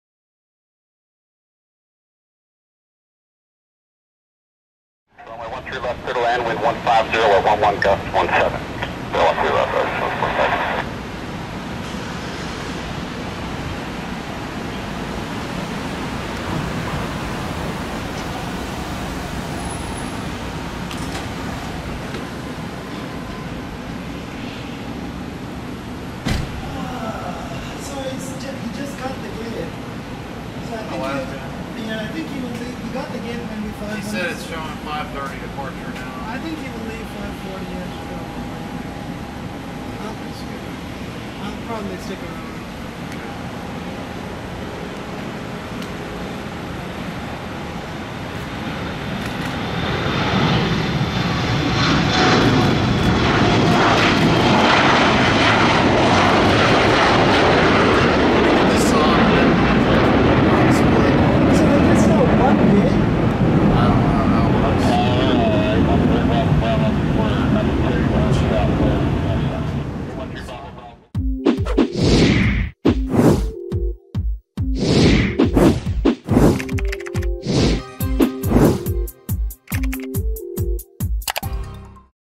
SkyWest Airlines ERJ-175/LR [N183SY] -- UHD 4K
SkyWest Airlines ERJ-175/LR departing on 13L from Dallas Love Field (DAL) on July 25, 2021